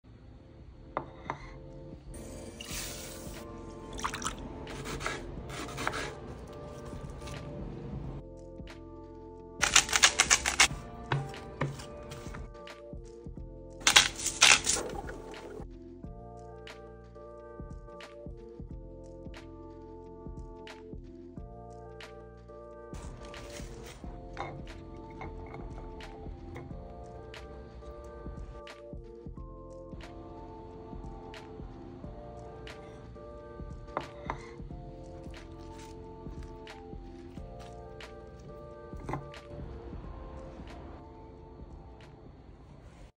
Vegan Wild Rice Bowls
This vibrant wild rice bowl is packed with nourishing ingredients like chickpeas, corn, pickled onions, and beets, all topped with a zesty herb lemon yogurt sauce for a healthy and delicious plant-based meal.